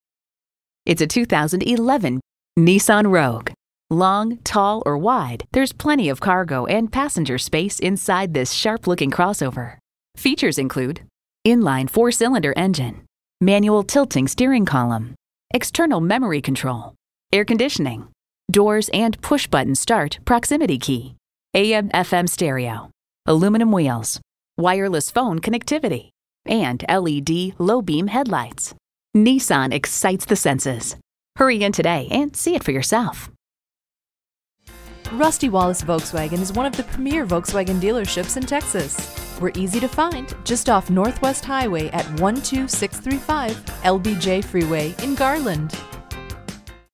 Used 2011 Nissan Rogue Dallas TX Garland, TX V200410B - SOLD
Year: 2011
Make: Nissan
Model: Rogue
Trim: SV
Engine: 2.5L I4 DOHC 16V
Transmission: CVT
Color: Black
Interior: gray
Mileage: 107751
Stock #: V200410B
VIN: JN8AS5MT1BW571114
Super Black Metallic 2011 Nissan Rogue SV FWD CVT 2.5L I4 DOHC 16V Rogue SV, 2.5L I4 DOHC 16V, CVT, FWD, Super Black Metallic, gray Cloth.brbrRecent Arrival! 22/28 City/Highway MPG

Address:
12635 Lyndon B Johnson Freeway